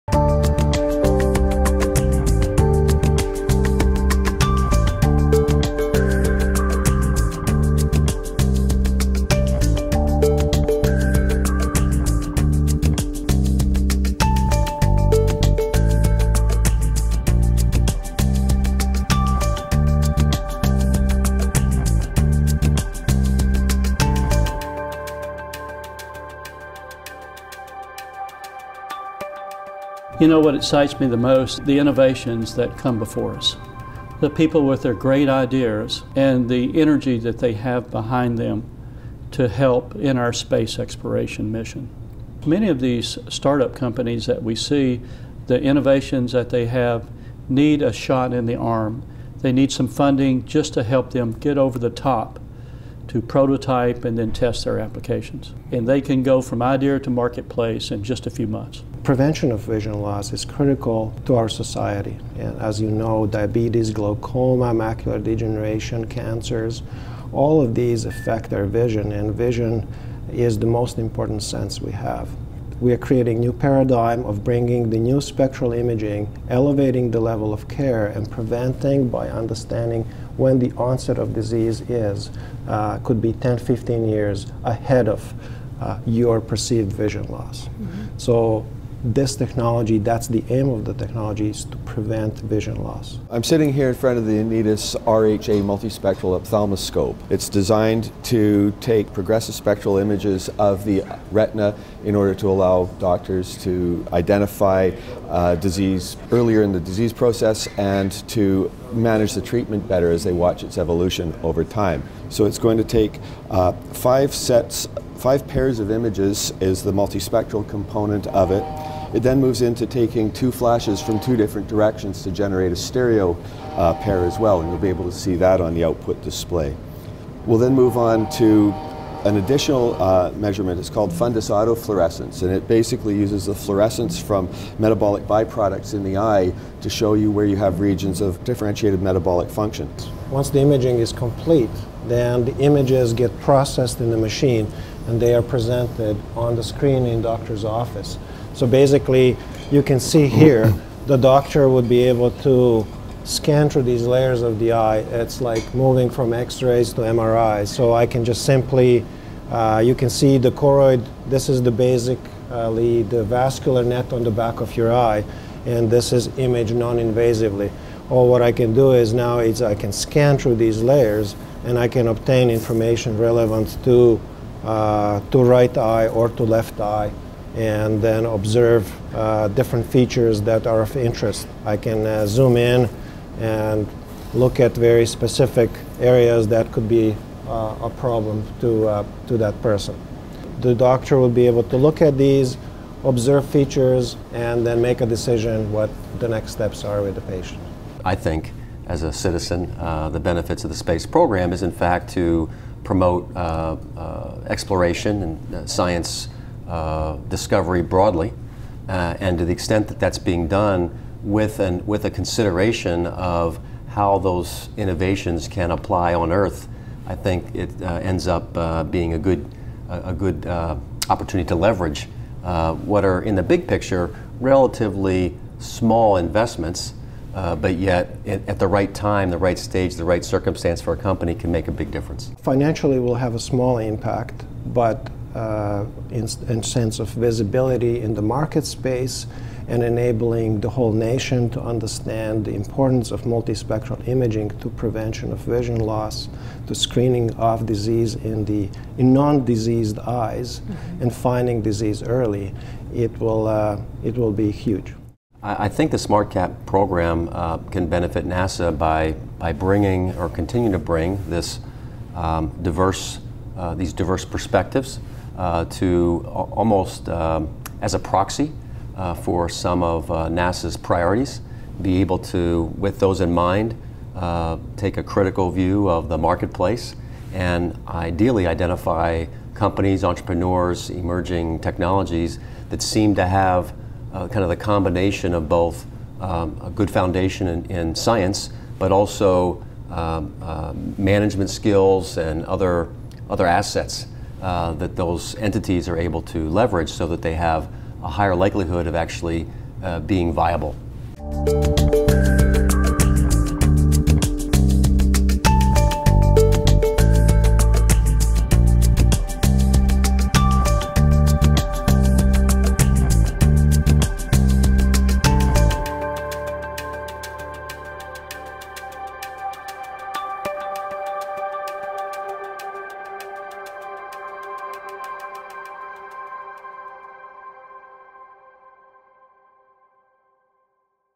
Annidis v3
SMARTCAP grant recipient Annidis demonstrates their revolutionary technology for early detection of outer retinal and choroidal disorders, which can potentially affect astronauts during long duration space travel.